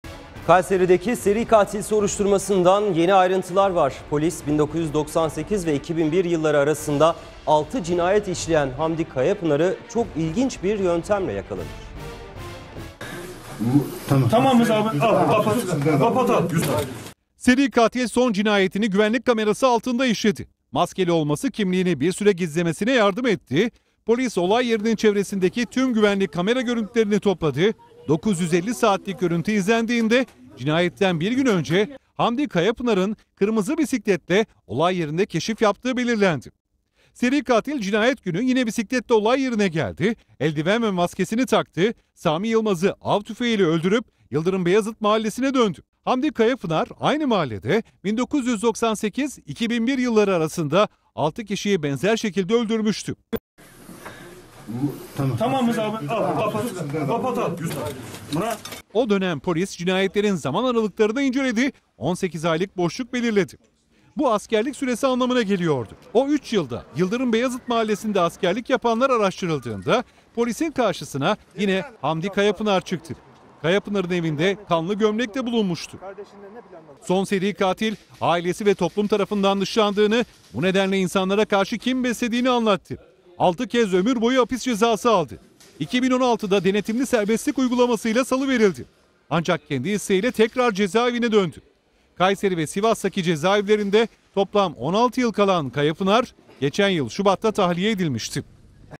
Seri Katili Askerlik Detayı Yakalattı
Kayseri’deki seri katil soruşturmasından yeni ayrıntılar geliyor. Polis, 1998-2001 yılları arasında 6 cinayet işleyen Hamdi Kayapınar’ı, çok ilginç bir yöntemle yakalamış.

TGRT HABER TV HAKKINDA:

● Türkiye'nin kanalıyız...